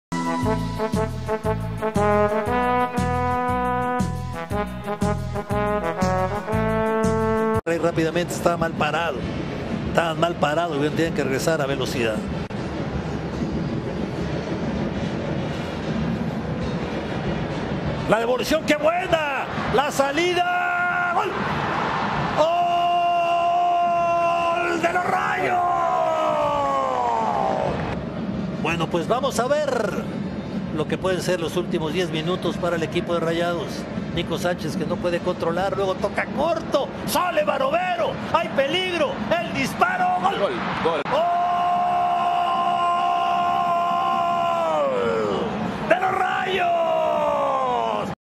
Rayados VS Necaxa - J9 Liga BBVA Mx
Monterrey y Necaxa se enfrentan en el Estadio BBVA; Rayados Cae 0-2 frente a los Hidrocalidos